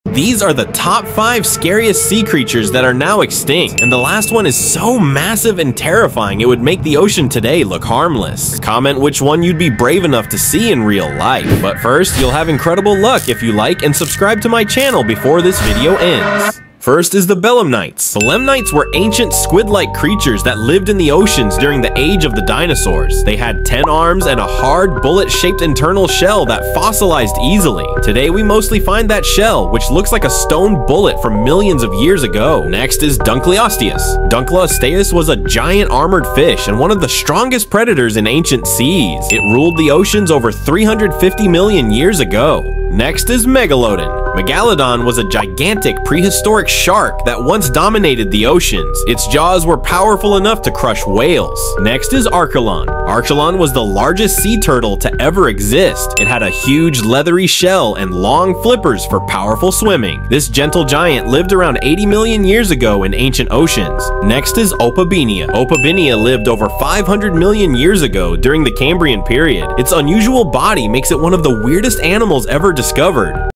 Top 5 Sea Creatures that are now extinct
Dive into the terrifying world of extinct sea creatures that once ruled our oceans. From the massive Megalodon to bizarre ancient predators like Dunkleosteus and Opabinia, these long-lost animals were unlike anything alive today. In this video, we reveal their most shocking characteristics, unique behaviors, and why they disappeared forever.
If you’re fascinated by ocean mysteries, prehistoric monsters, and rare extinct species, this is the video for you.
Don’t forget to comment which creature surprised you the most!